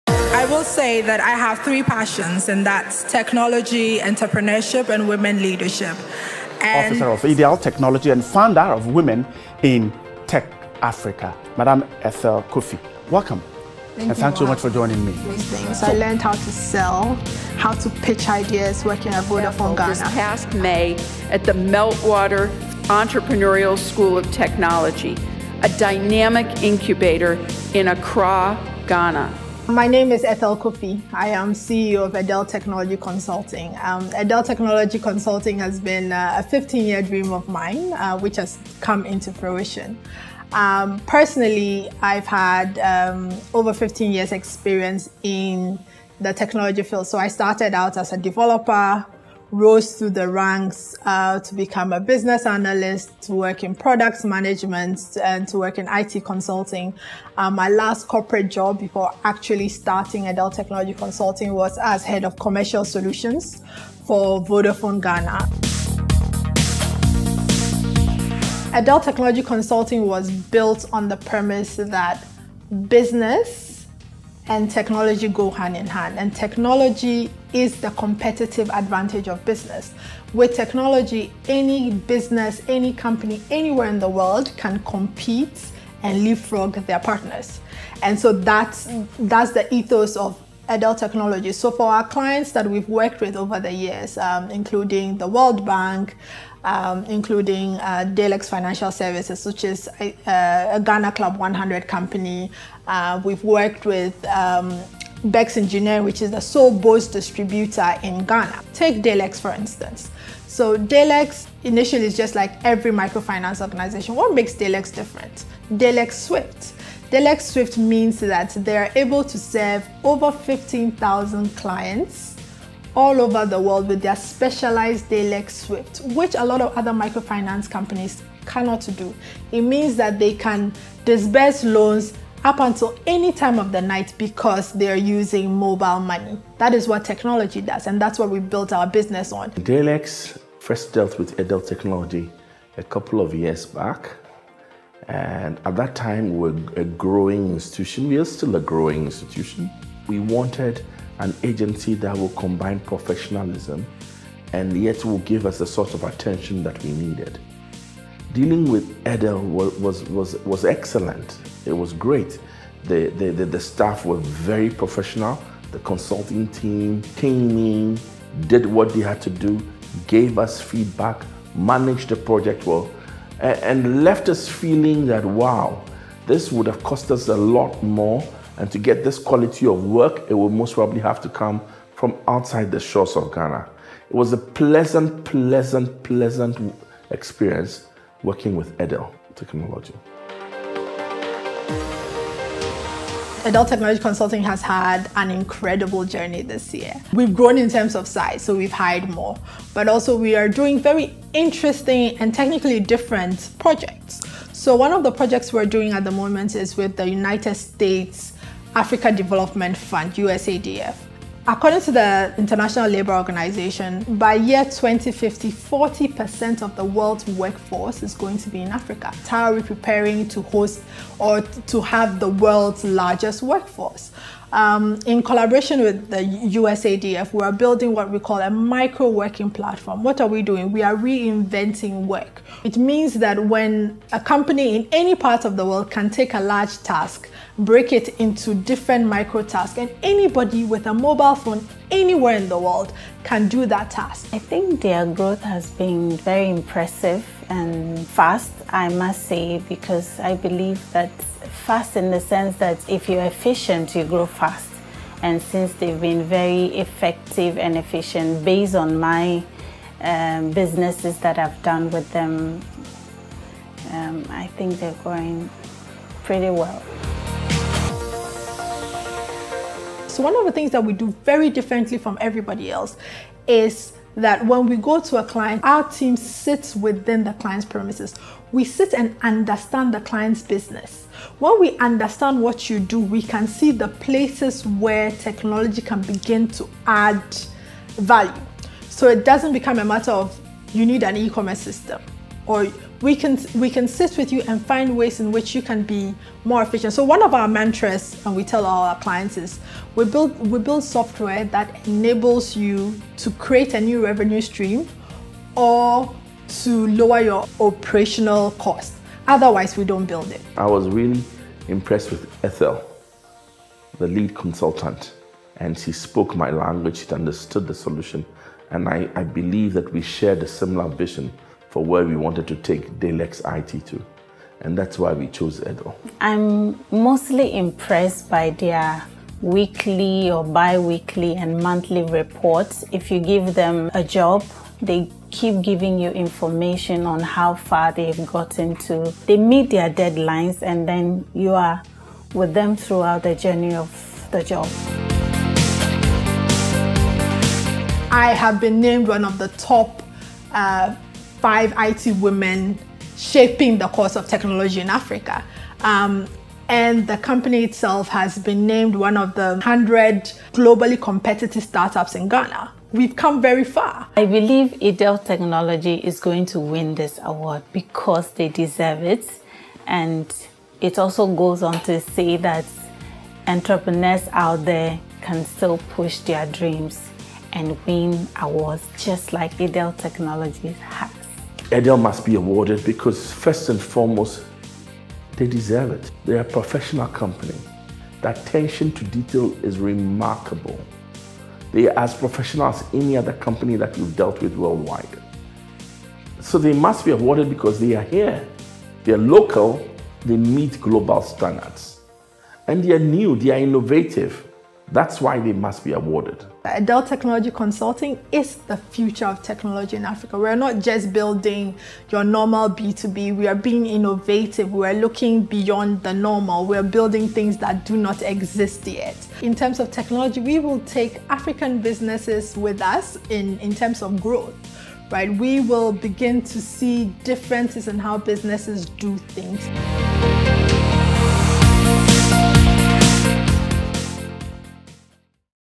EDEL Technology Consulting : IT Consulting Firm of the Year Ghana IT and Telecom Awards
The CEO  of EDEL Technology Consulting and a select number of Clients talk about why EDEL is the IT Consulting Firm of the Year